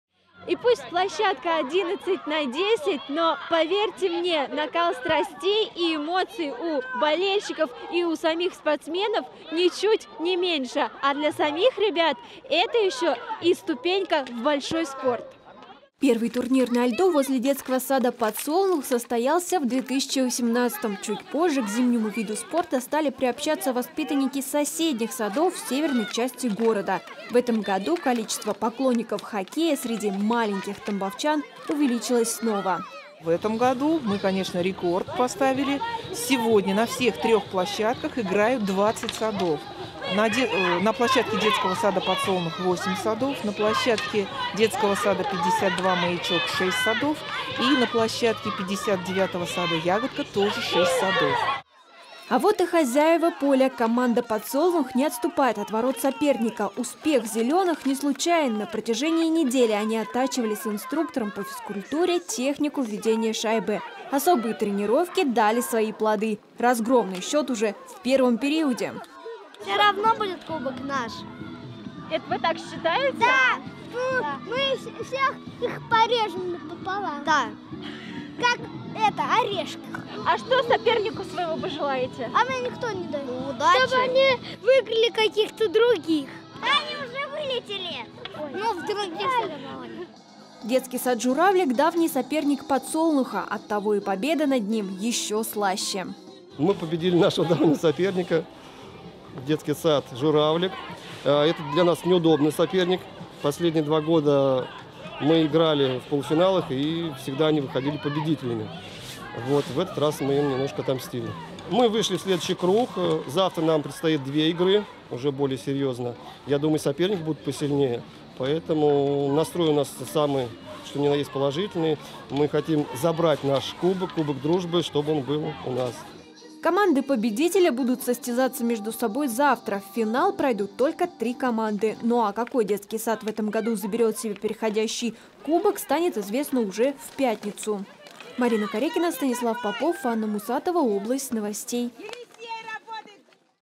В Тамбове в детском саду "Подсолнух" стартовал ежегодный турнир по хоккею
Нешуточные спортивные баталии сегодня разыгрались в детском саду "Подсолнух". Там стартовал ежегодный турнир по хоккею. В этом году любителей этого вида спорта среди маленьких тамбовчан стало ещё больше. На трёх площадках за переходящий кубок чемпиона борются сразу 20 садов.